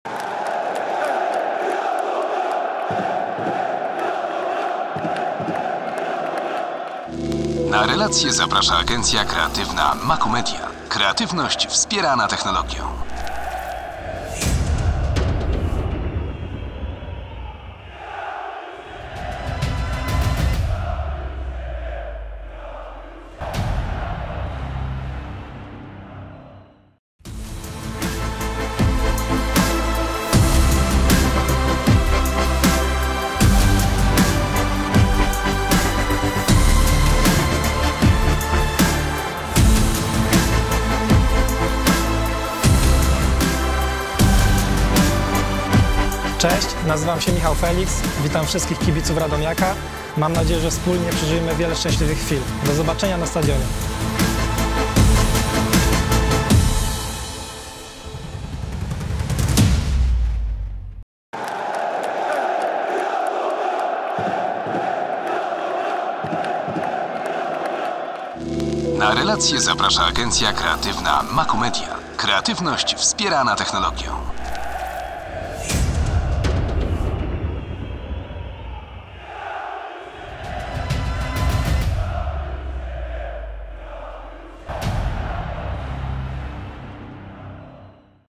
Michał Feliks piłkarzem Radomiaka! [RADOMIAK.TV]
Michał Feliks podpisał kontrakt z Radomiakiem Radom! Umowa z 23-latkiem obowiązywać będzie przez trzy lata. Ofensywny zawodnik przeszedł do naszego Klubu na zasadzie transferu definitywnego, gotówkowego z Garbarni Kraków.